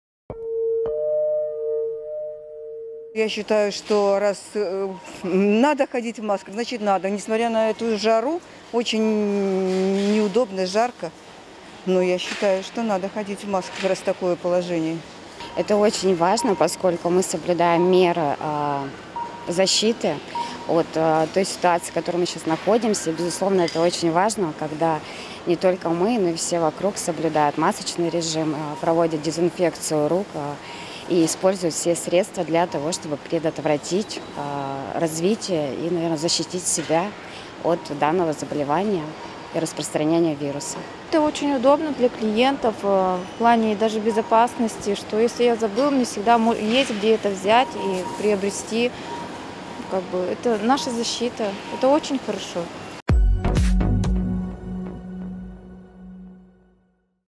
Покупатели активно пользуются средствами защиты
Посетителям торговых центров ТЦ "Сити Молл" и "Кристалл" предоставляют бесплатные одноразовые маски.

Информация о необходимости соблюдения масочного режима и других противоэпидемических мер транслируется в торговых залах через громкоговорители каждые 20 минут и размещена на специальных афишах при входе. Акция будут действовать до отмены масочного режима.

Предложение бесплатных средств индивидуальной защиты тюменцы считают актуальным. "Это очень удобно, если ты забыл взять маску с собой или по времени ее необходимо сменить. Можно не беспокоиться, а заняться непосредственно покупками", - поделилась мнением Мария Смирнова.

В связи с введением с 14 мая обязательного масочного режима сотрудники департамента потребительского рынка и туризма Тюменской области, Роспотребнадзора и других ведомств следят за наличием масок и других индивидуальных средств защиты в розничной и аптечной сетях, а также за ценами на эту группу товаров, в ежедневном режиме